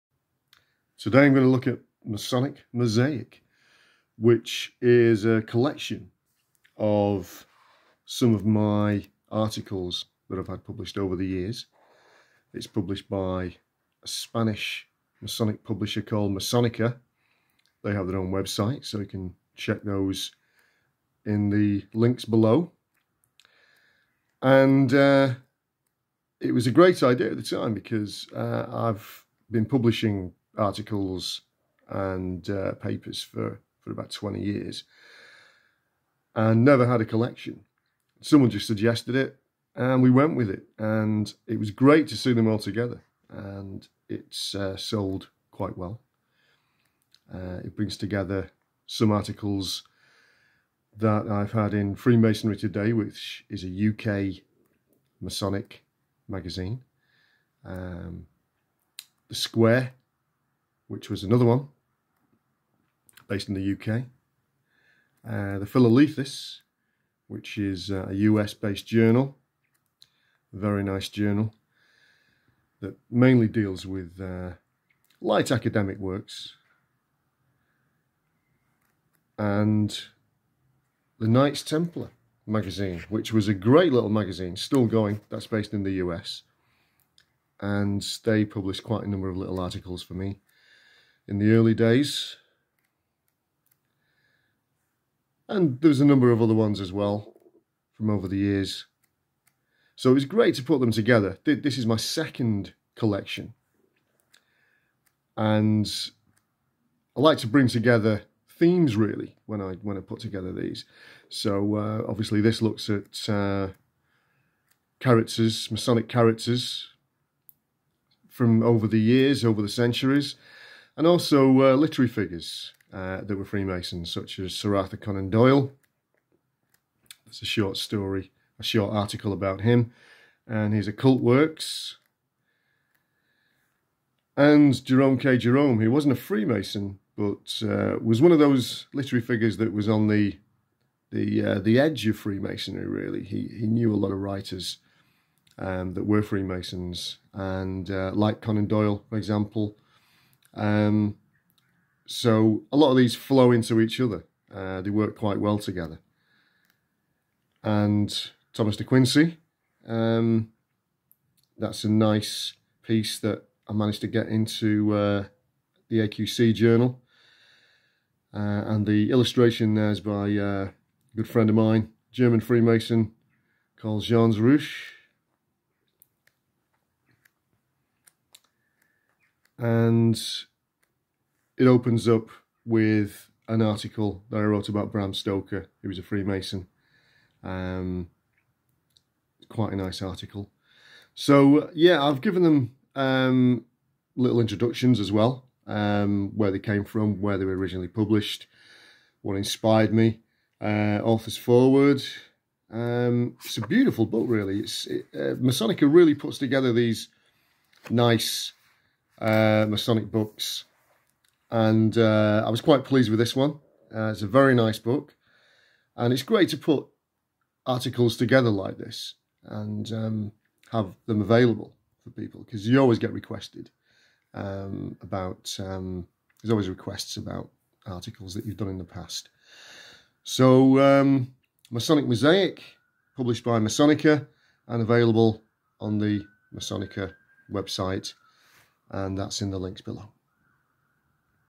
A look at my book Masonic Mosaic; the second collection of my Masonic articles and essays
A look at my book Masonic Mosaic, which is published by the Spanish Masonic publisher Masonica. the book is the second collection of my articles and papers, including pieces on Bram Stoker, Sir Arthur Conan Doyle and Dr Jean Theophilus Desaguliers.